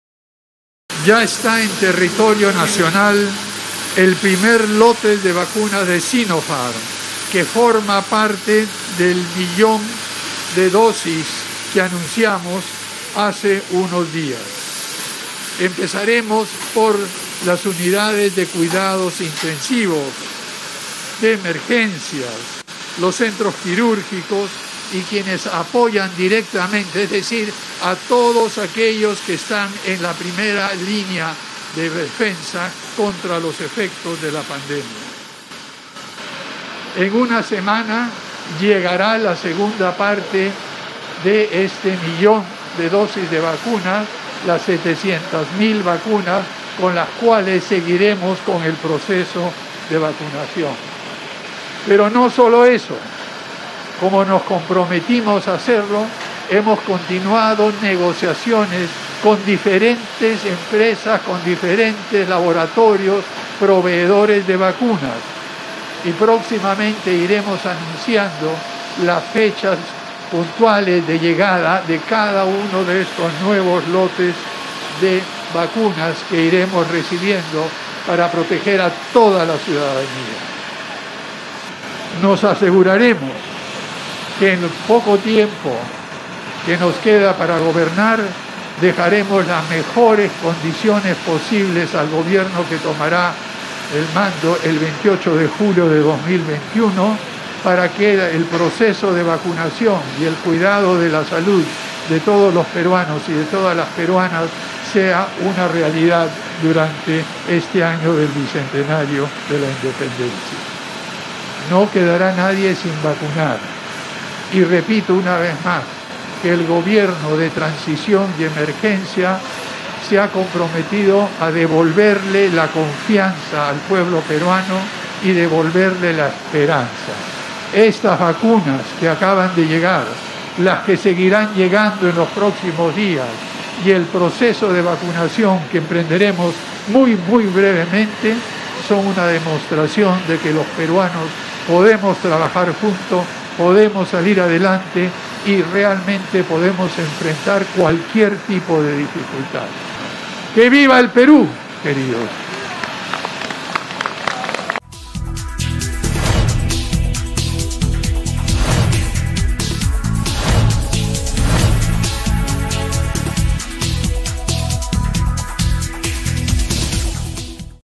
MENSAJE DEL PRESIDENTE FRANCISCO SAGASTI SOBRE LLEGADA DE VACUNAS COVID 19
MENSAJE A LA NACIÓN DEL PRESIDENTE DE LA REPÚBLICA FRANCISCO SAGASTI HOCHHAUSLER 07 DE FEBRERO DE 2021
Estimados compatriotas: Conforme hemos venido anunciando, ya está en territorio nacional el primer lote de vacunas de Sinopharm, que forma parte del millón de dosis que anunciamos hace unos días. A diez días de cumplir tres meses del gobierno de transición y de emergencia, estamos cumpliendo con el compromiso que asumimos en todo el país de luchar contra la pandemia de la covid-19, obteniendo la vacuna que es el mejor escudo de protección que tenemos para cuidarnos a nosotros y a todos los peruanos. Ya tenemos este primer lote de 300,000 dosis que en muy poco tiempo empezarán a administrarse en todo el territorio nacional. Empezaremos por las unidades de cuidados intensivos, emergencia, centros quirúrgicos y quienes apoyan directamente, es decir a todos los que están en la primera línea de defensa contra los efectos de la pandemia. Este es el comienzo de una tarea larga y difícil, que no solamente es un desafío para nuestro país, sino que en la actualidad es un desafío para todo el mundo. Hemos sido testigos de los problemas que han enfrentado diferentes países, no solo de la región, sino países en Europa, Norteamérica, para poder obtener la vacuna, distribuirla y aplicarla; pero nosotros hemos venido avanzando a paso firme y con tranquilidad, demostrando que somos un gobierno que dice la verdad y las cosas como son. En una semana llegará la segunda parte de este millón de vacunas, las 700,000 dosis con las cuales seguiremos el proceso de vacunación. Pero no solo eso, como nos comprometimos hacerlo, hemos continuado negociaciones con las diferentes empresas, laboratorios, proveedores de vacunas, y próximamente iremos anunciando la fecha puntual de la llegada de vacunas de estos lotes que iremos recibiendo para proteger a toda la ciudadanía.
Quiero agradecer muy especialmente al equipo peruano que ha negociado durante muchísimo tiempo y sin descanso para obtener este primer lote de un millón de vacunas de Sinopharm, que como saben ustedes forma parte de un compromiso mucho mayor de provisión de vacunas. Agradecemos a la empresa Sinopharm que a lo largo de las negociaciones ha demostrado su buena voluntad y también al gobierno de la República Popular China, que, con el apoyo administrativo, el apoyo en los trámites necesarios, ha permitido que estas 300 mil vacunas que ven ahora lleguen incluso antes de lo que habíamos programado. Tengo también que agradecer, no sólo a los ministros de Relaciones Exteriores; de Economía y Finanzas; sobre todo a la Ministra de Salud que, sin desmayo y sin descanso, ha venido lidiando y liderando estas negociaciones desde el punto de vista de salud. Tal como lo ha hecho desde el punto de vista diplomático, nuestra Canciller; y, desde el punto de vista económico, nuestro Ministro de Economía y Finanzas. Todos ellos, coordinados por la Presidente del Consejo de Ministros, han venido trabajando sin descanso. Pero no solo ellos, no es sólo el gobierno que ha participado en esta tarea de hacernos contar con las vacunas que tenemos ahora en suelo peruano. Agradecemos la decidida participación del sector privado en múltiples formas, no solamente con el apoyo financiero para el transporte, sino también con el apoyo logístico, con la experiencia y la capacidad de gestión en que ha venido colaborando. Y los instamos a seguir haciendo esto, no solo en ese campo, sino en otros también y, particularmente, también en la provisión de oxígeno. Les pedimos a las empresas industriales, mineras y de hidrocarburos que, ante esta terrible escasez de oxígeno que tenemos en la actualidad, continúen colaborando de la manera extraordinaria como lo han venido haciendo, y pongan a disposición de los hospitales y diferentes centros médicos, la provisión de oxígeno que ahora es destinada a la producción industrial y minera. Les pedimos, por unos días, de desviar de producir minerales e hidrocarburos hacia salvar vidas, es lo que necesitamos en la actualidad. También, en el proceso de administrar las vacunas, agradecemos el compromiso de los Gobiernos Regionales, de la Policía Nacional y las Fuerza Armadas, que nos permitirá contar con todo el apoyo logístico para que la vacuna llegue hasta los rincones más alejados de nuestro país.
Por último, y no por ello menos importante, tenemos que agradecer todos los peruanos a esas incansables madres de familia que han venido colaborando en los comedores populares y ollas comunes; a esas personas que, generosamente, han compartido los víveres, su tiempo y esfuerzo, para aliviar el sufrimiento de muchísimos peruanos y peruanas. Queda ahora, realmente, una tarea muy grande por delante. Esta tarea que nos queda es continuar con todos los esfuerzos que estamos haciendo a nivel del país, para trabajar conjuntamente.